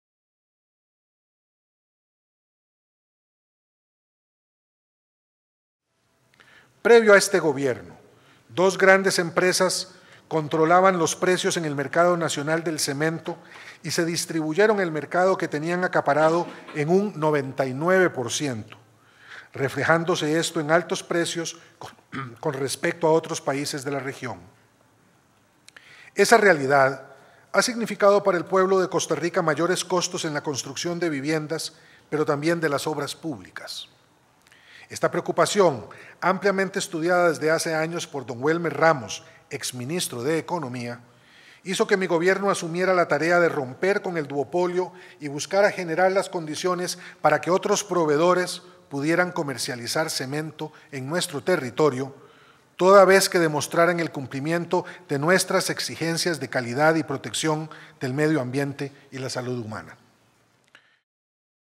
2: "Mi Gobierno asumió la tarea de romper el duopolio del cemento"
Mensaje del Presidente de la República Luis Guillermo Solís durante la comparecencia ante la Comisión Legislativa investigadora del tema de los créditos del cemento.